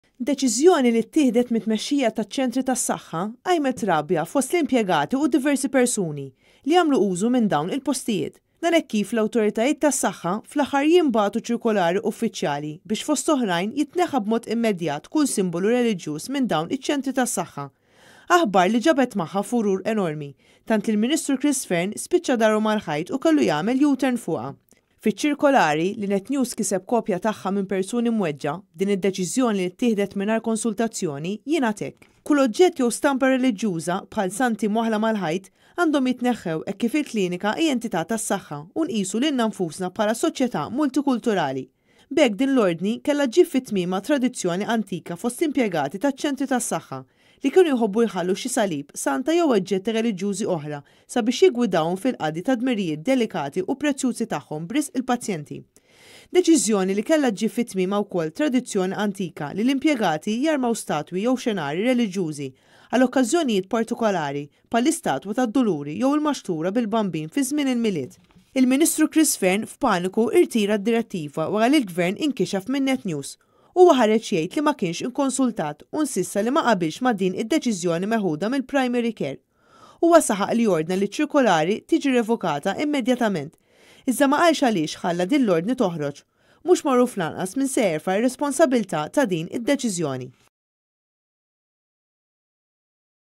Rieud jneħħu s-Salib u kull simbolu reliġjuż miċ-Ċentri tas-Saħħa - Chris Fearnema kienx ikkonsultat
Deċiżjoni li ttieħdet mit-tmexxija taċ-Ċentri tas-Saħħa (il-bereġ) qed tqajjem rabja fost l-impjegati u wkoll diversi persuni li jagħmlu użu minn dawn il-postijiet.